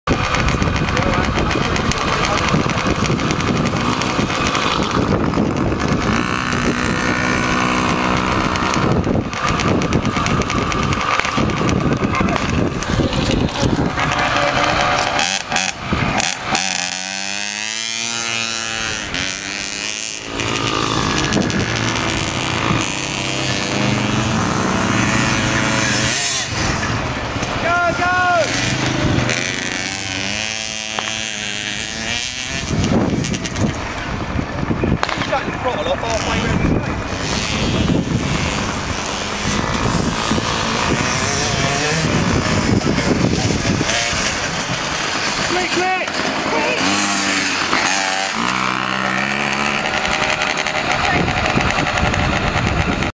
a 7 year old lad at bonville farm, on his ktm 50... very good!!
just a short video of a 7 year old lad, who was going around the junior track, very quick. 30sec's lap time, hope he has a good future in mx.. :)